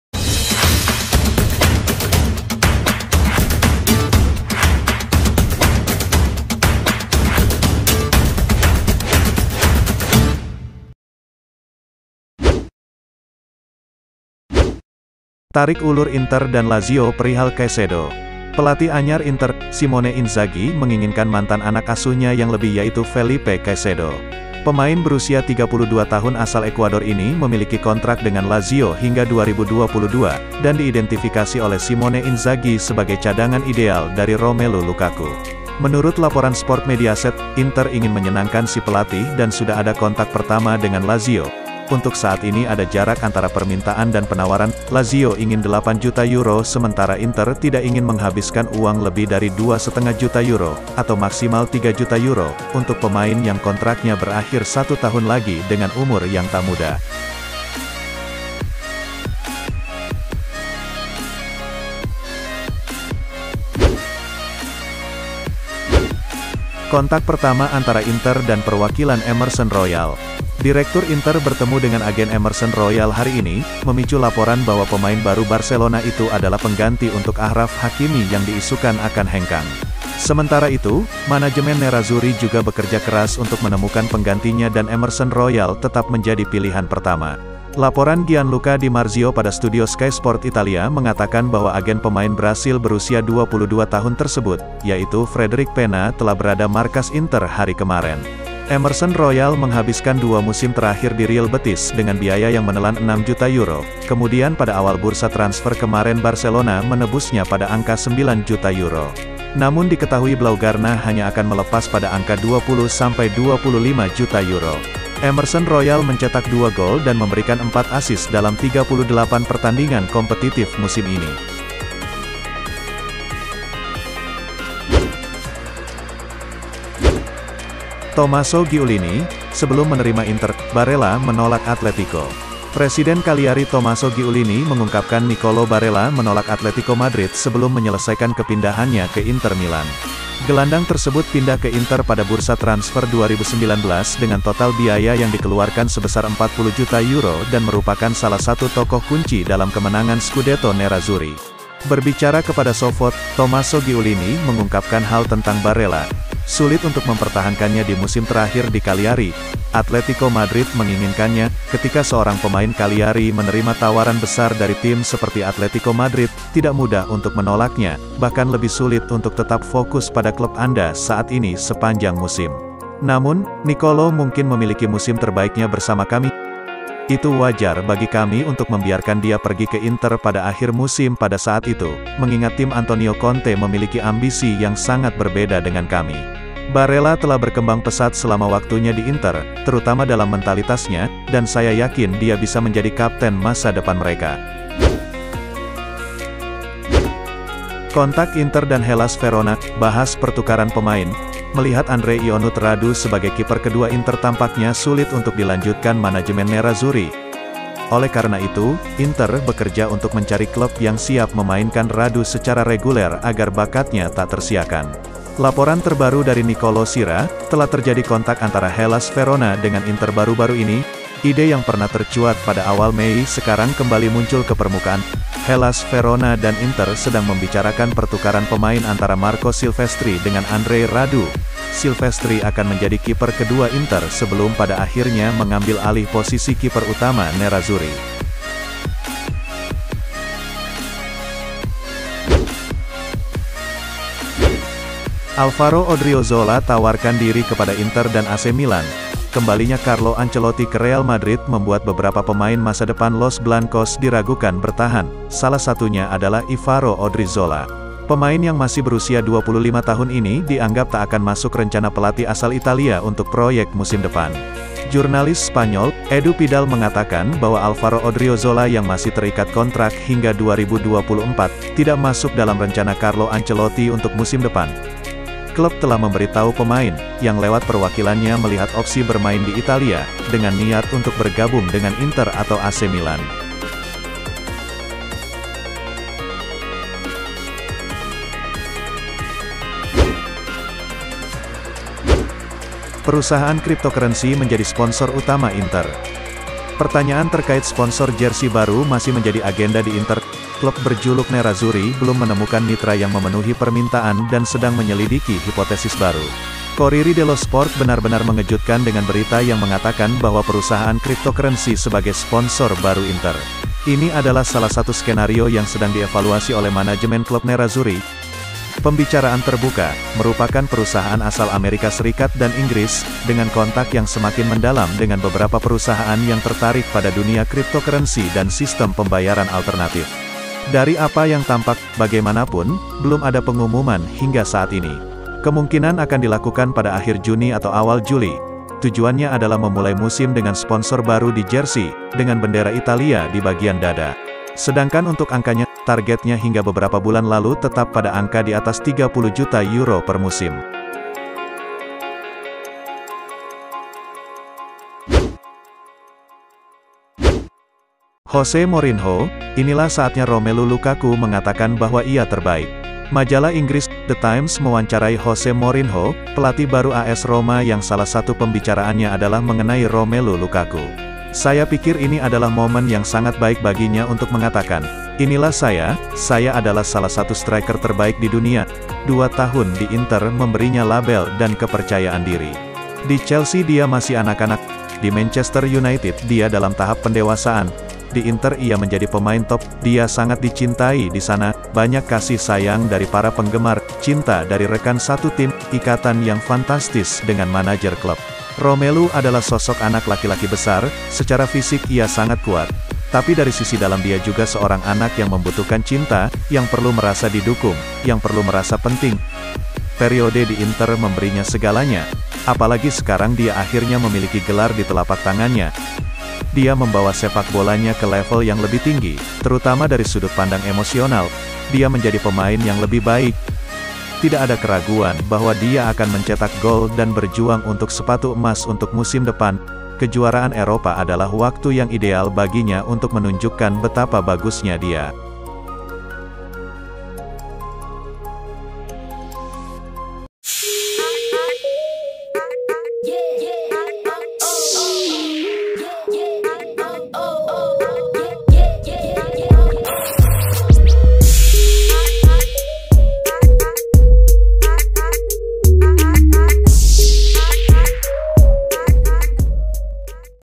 Sponsor pengganti 26 thn pireli 😥 update transfer inter 😗 bajak madrid barca ❗berita inter milan
Berita Inter milan terbaru hari ini :

🔵Pemain baru barcelona, emerson royal jadi target transfer gantikan peran achraf hakimi
🔵 Barella pernah tolak atletico madrid
🔵 Inter dan hellas verona bahas transfer ionut Radu
🔵Pemain madrid, alvaro odriozola ingin ke inter 🔵Perusahaan crypto calon sponsor inter 🔵Mourinho : Lukaku yang terbaik